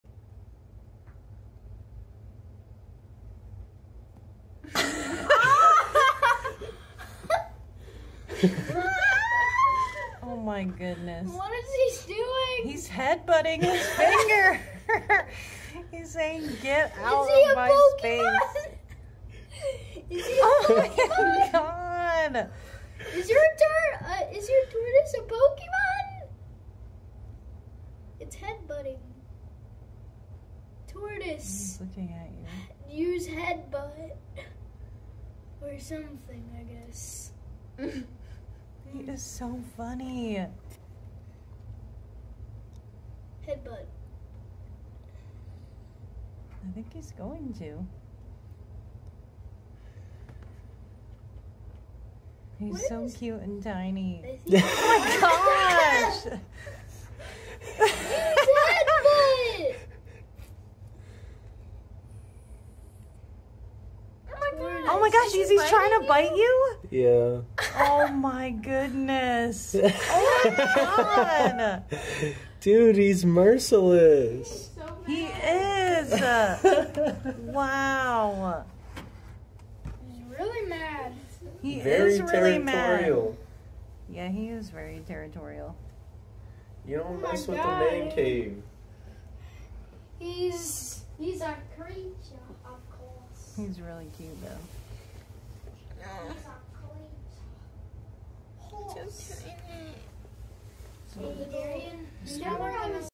Baby Greek tortoise head butting 😂
My son has a baby Greek tortoise. I went to visit him and he mentioned that it head butts him, so I asked him to show me 😂